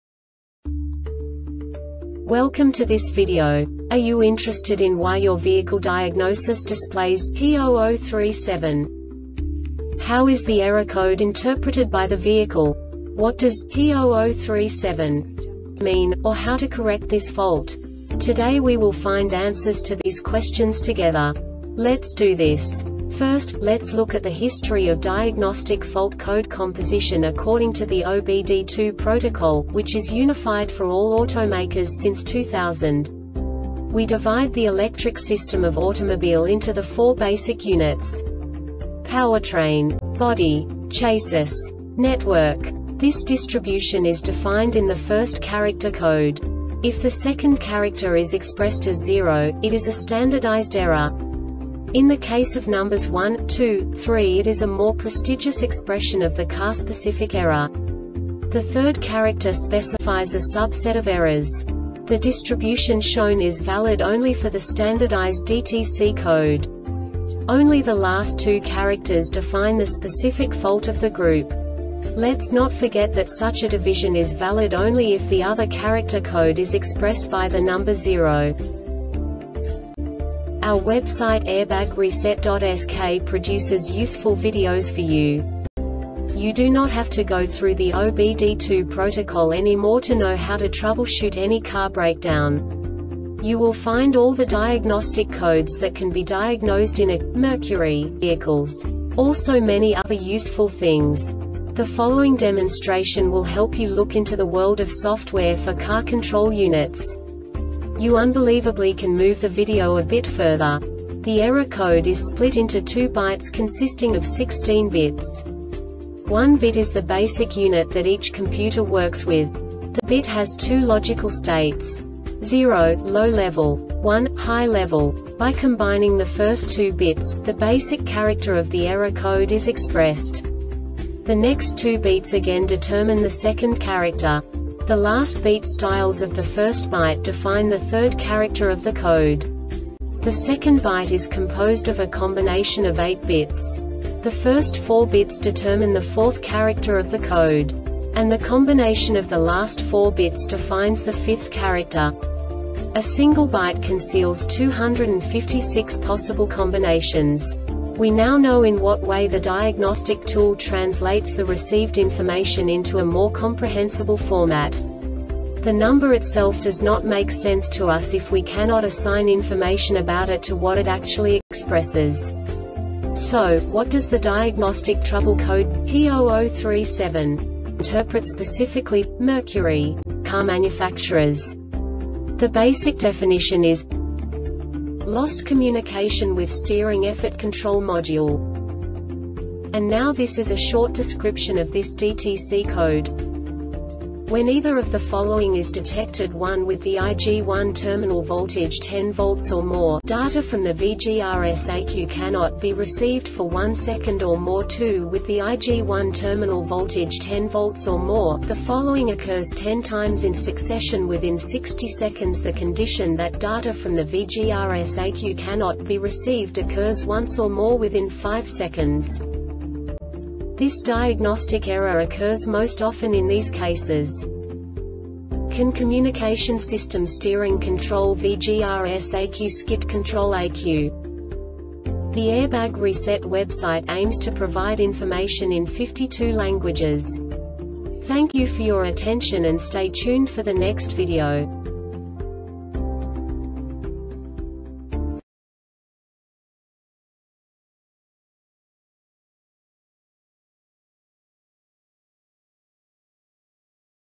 DTC Mercury P0037 Short Explanation
Contents:
0:21 Basic DTC analysis according to OBD2 protocol standard.
1:48 Insight into programming
2:58 A diagnostically understandable description of the Diagnostic Trouble Code
3:05 Basic error definition

Make
Mercury
Code
P0037
Definition
Lost Communication with Steering Effort Control Module
Description
When either of the following is detected 1 With the IG1 terminal voltage 10 V or more,  data from the VGRS ECU cannot be received for 1 second or more  2 With the IG1 terminal voltage 10 V or more,  the following occurs 10 times in succession within 60 seconds   The condition that data from the VGRS ECU cannot be received occurs once or more within 5 seconds
Cause
 CAN communication system Steering control VGRS ECU Skid control ECU